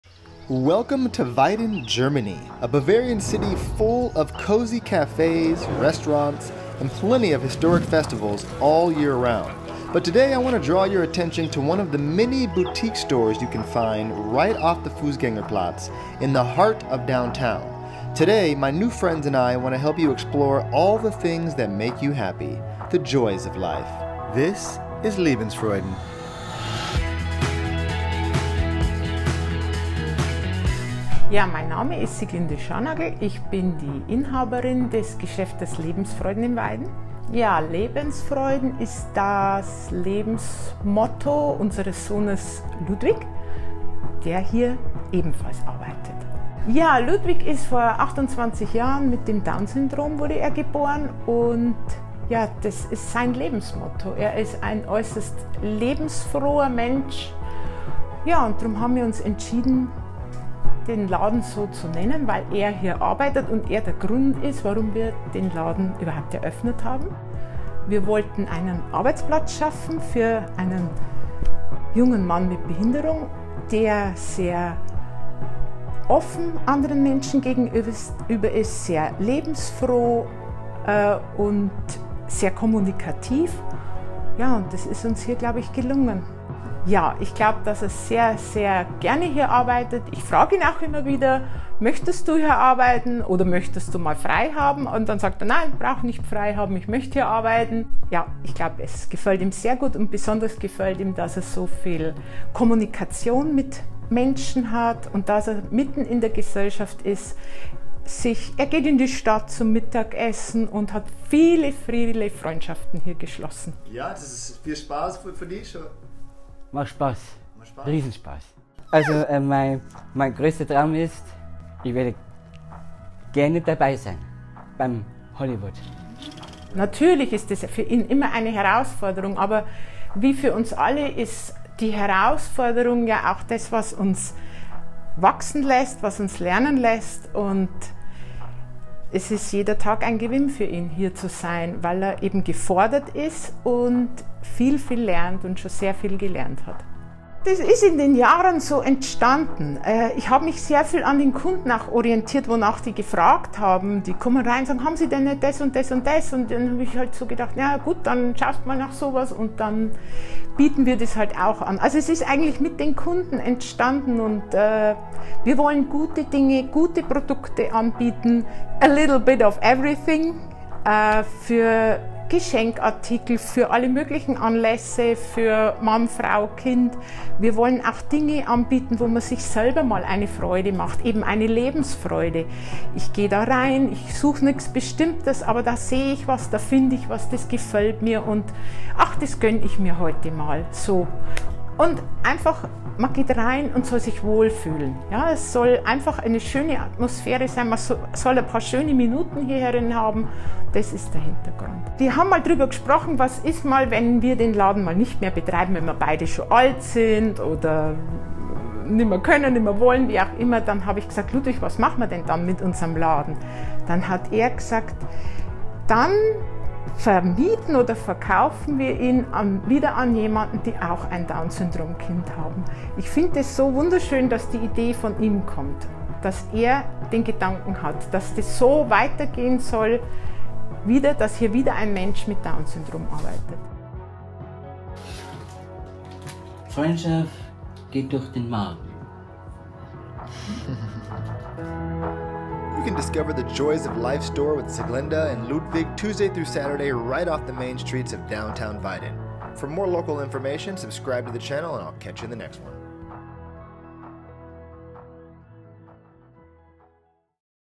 What are shops like in Weiden Germany? A look into "Lebensfreuden" and working with a disability
Welcome to Weiden Germany, a Bavarian city full of cozy cafes, restaurants, and plenty of historic festivals all year around, but today I want to draw your attention to one of the many boutique stores you can find right off the fußgänger Platz in the heart of downtown.  Today my new friends and I want to help you explore all the things that make you happy, the joys of life, this is Lebensfreuden.

Owner Sieglinde Scharnagl opened up Lebensfreuden aka “Joys of Life,” which was named after the life motto of her son, Ludwig, who also works there.

Ludwig was born with down syndrome, but that doesn’t stop him from being extremely cheerful and friendly to everyone around him.  Sieglinde wanted to create an opportunity for Ludwig to have a “normal” challenging job that could provide him with life skills, help him grow, and of course, give him a chance to make friends along the way.

Inside the store you can find all types of “make you feel good” products that change with the season.

To learn more about their store you can visit their

Address: Hinterm Zwinger 17, 92637 Weiden in der Oberpfalz

Hours of Operation:
Tues - Fri 10am - 6pm